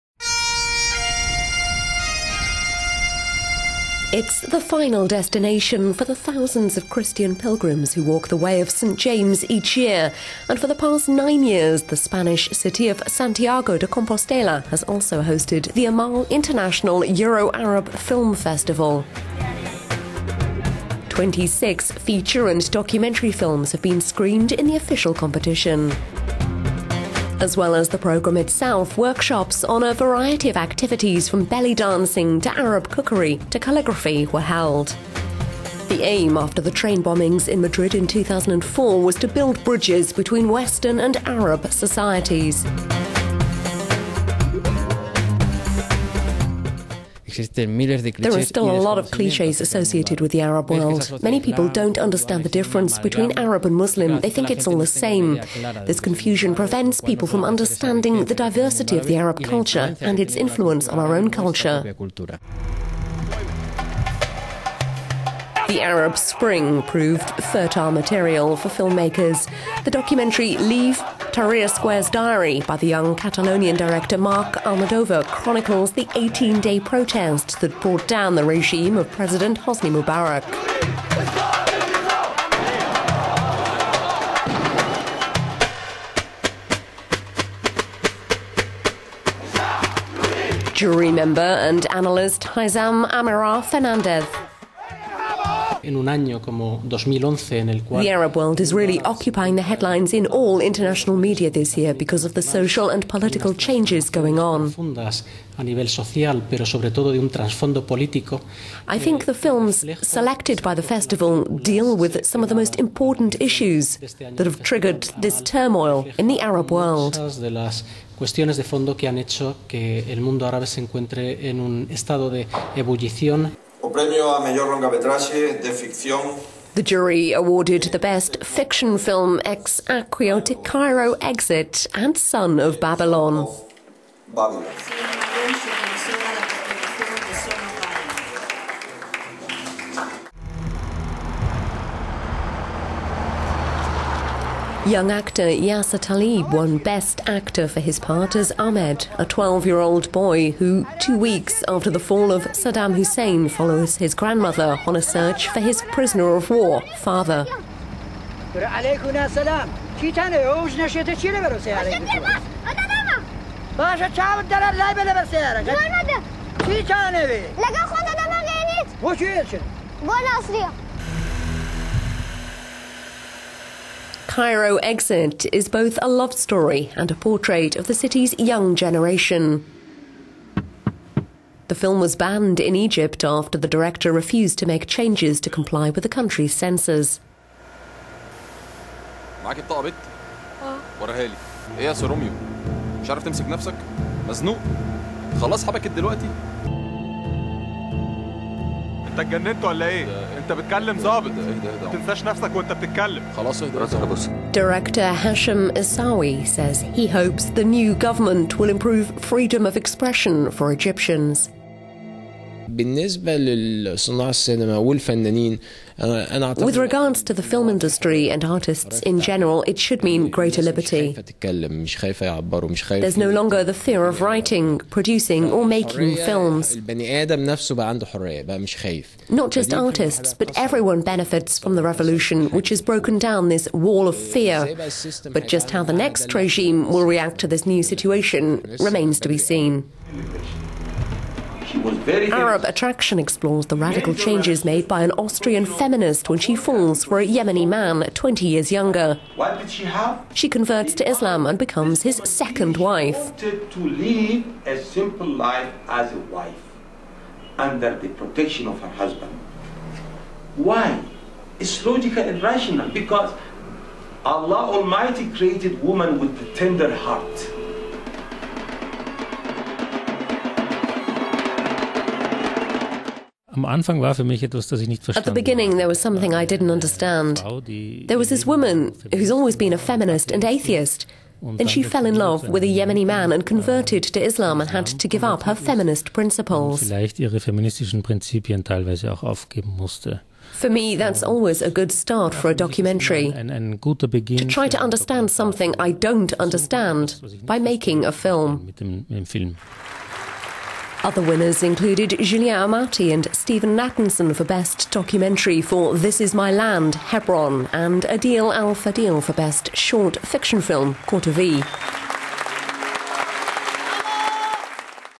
euronews cinema - Amal: a film festival of hope
It is the final destination for the thousands of Christian pilgrims who walk the Way of St James each year, and for the past nine years the Spanish city of Santiago de Compostela has also hosted the Amal International Euro-Arab Film Festival.